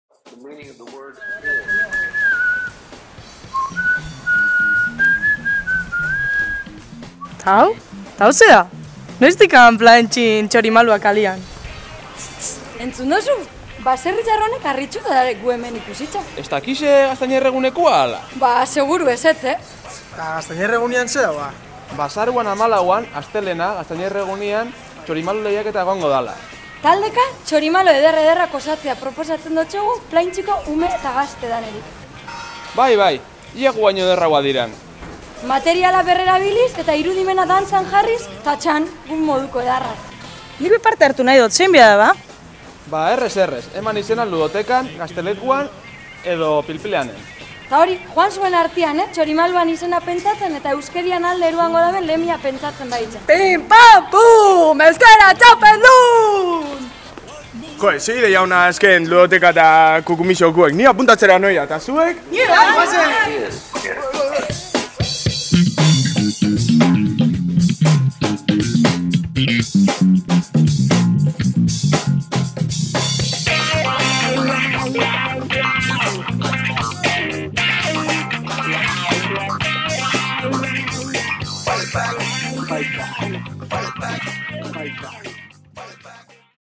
Gaztañerre 2016, Txorimalo lehiaketa kukumixorekin
• Taldekako txorimalo txapelketia!
• Sarixak parte hartzaile danendako ta giro bikaina jolasian!
• Material berrerabilgarrixa biltzen hasi, irudimena dantzan ipini!
• Txorimaluan izena eta euskerian aldeko lemia aukeratziari ekin!